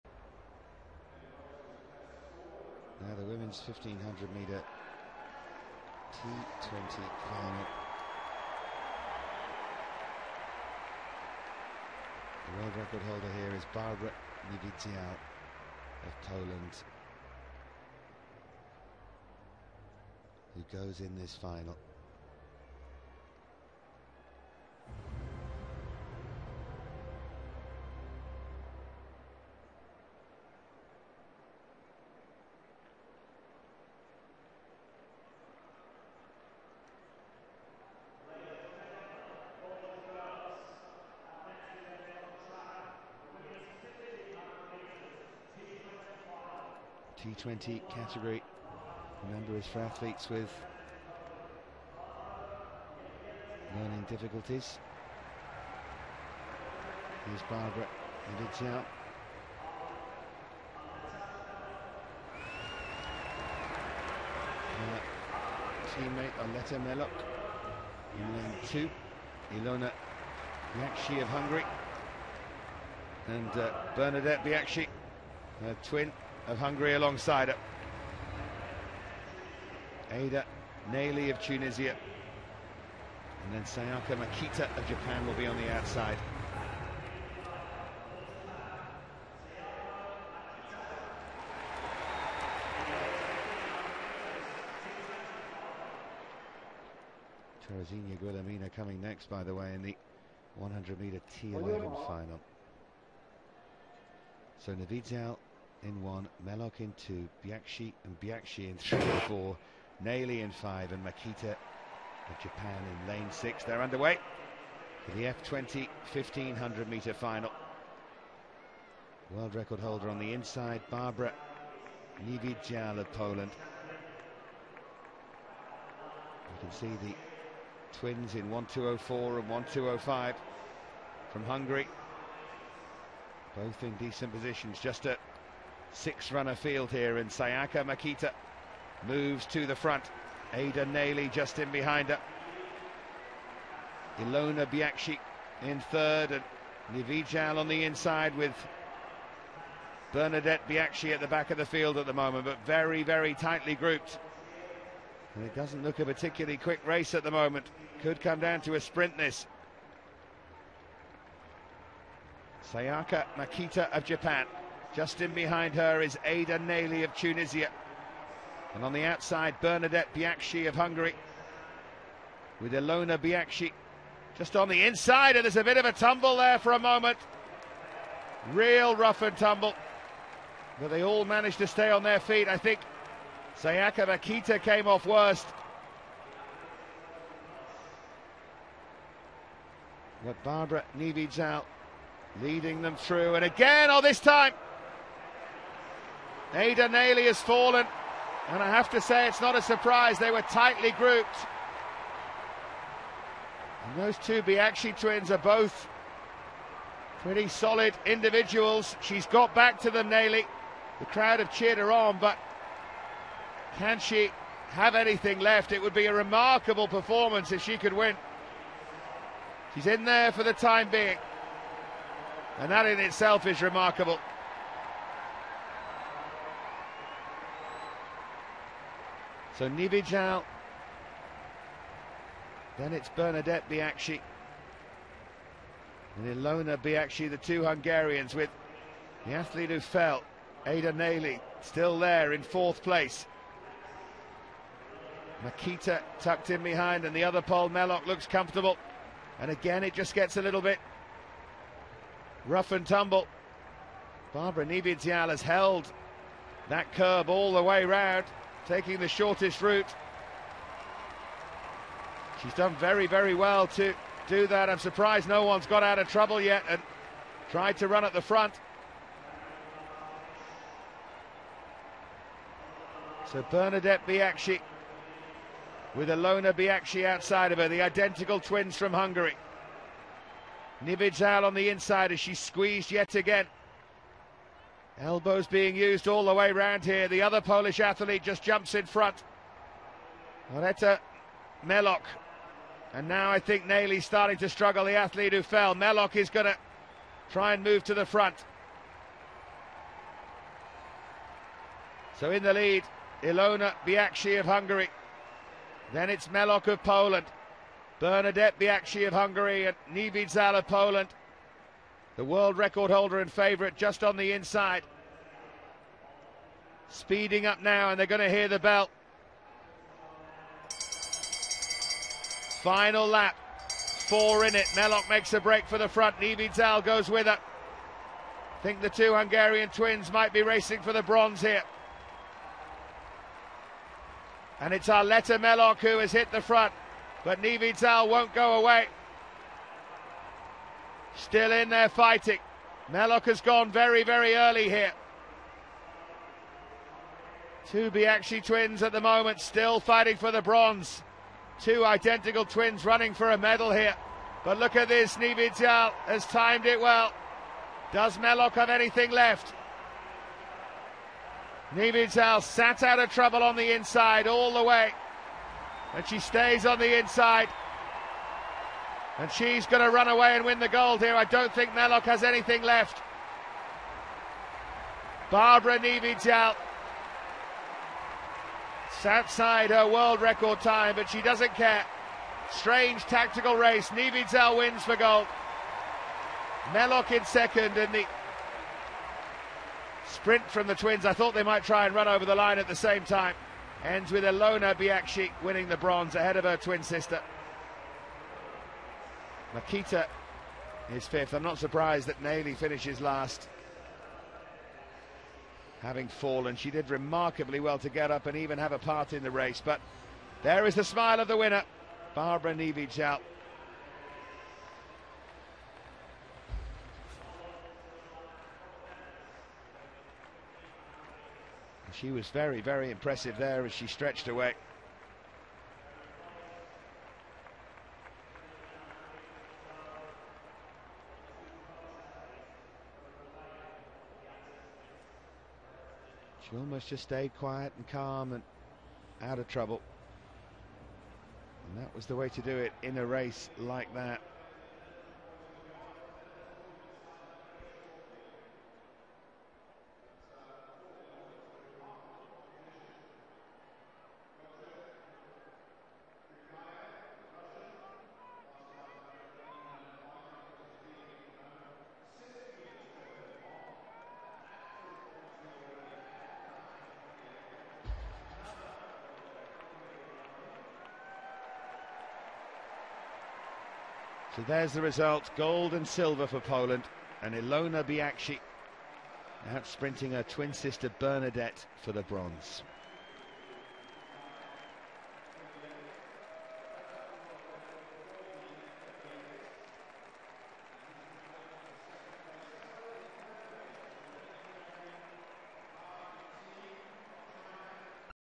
Barbara Niewiedzial i Arleta Meloch 1500m Paraolimpiada Londyn 2012
Barbara Niewiedzial i Arleta Meloch 1500m Paraolimpiada Londyn 2012. Bieg finalowy zloty i srebrny medal Polek